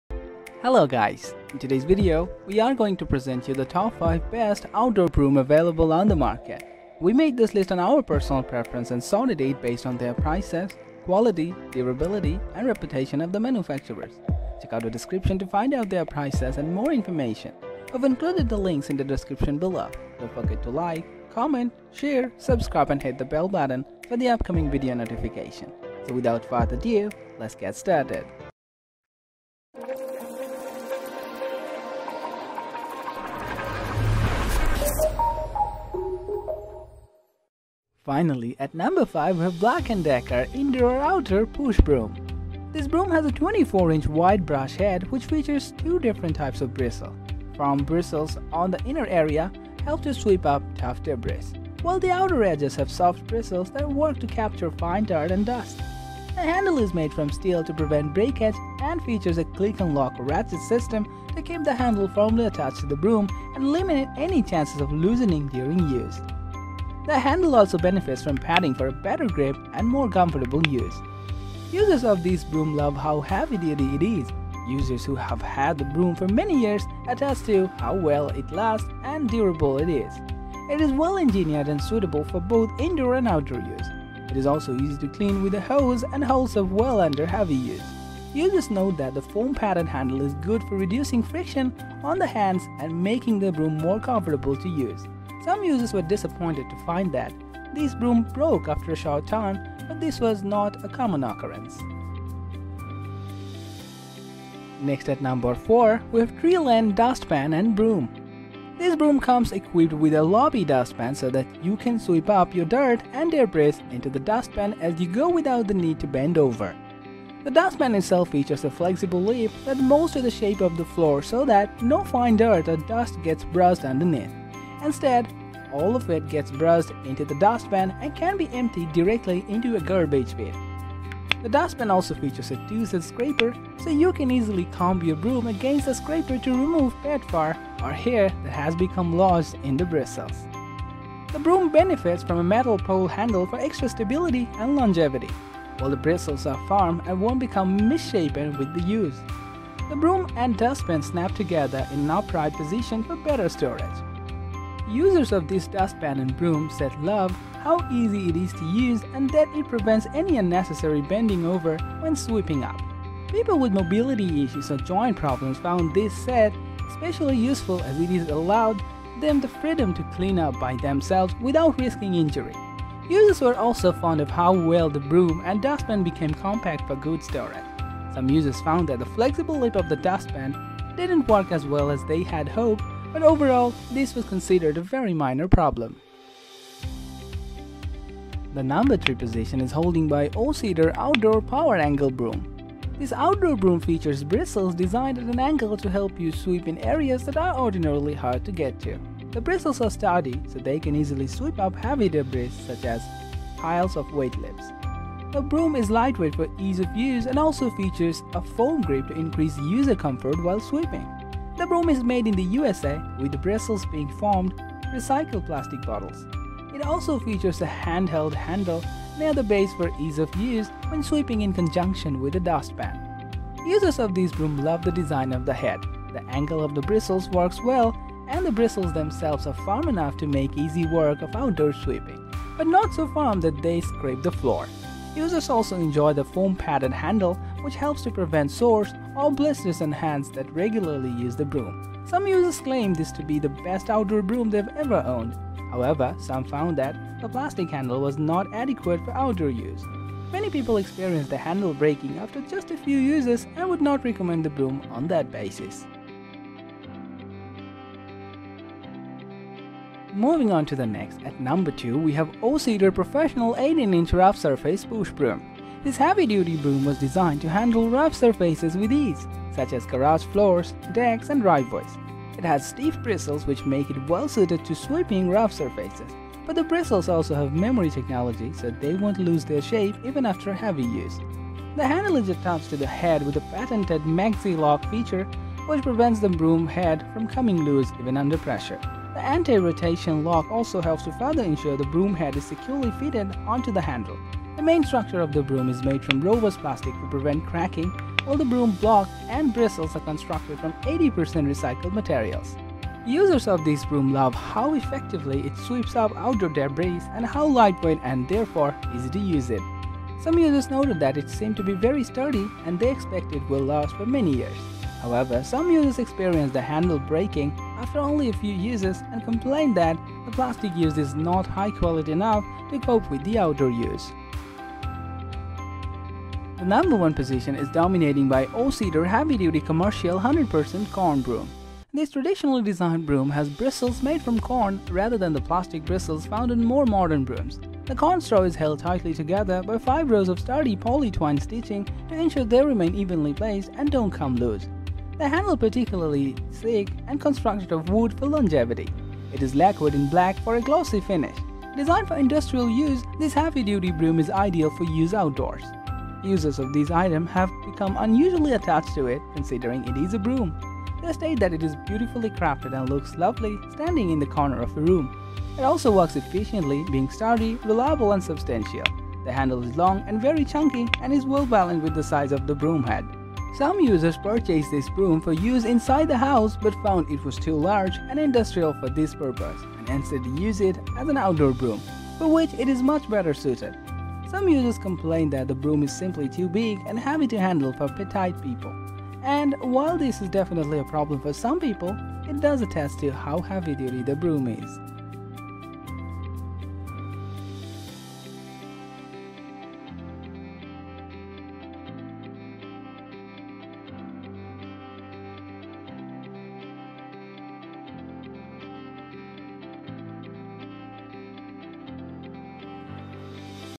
Best Outdoor Broom Reviews 2024 | Best Budget Outdoor Brooms (Buying Guide)
Are you searching for the Outdoor Brooms of 2024? These are the Top 5 Outdoor Brooms we found so far:

►5. Black & Decker 261086:
►4. TreeLen Dust Pan and Broom:
►3. O-Cedar Angle Broom 159833:
►2. O-Cedar Professional 18":
►1. O-Cedar Heavy Duty Com.:

To narrow down your searching effort, we have researched the market on the Outdoor Brooms. We already spent hours analyzing these top 5 Outdoor Brooms to ensure your worth buying.They all come with excellent features with a great price range.

This video on the Outdoor Brooms reviews in 2024 will add value for the money. So keep watching till the end and select the suited one for you.

Visit Other Shoes Videos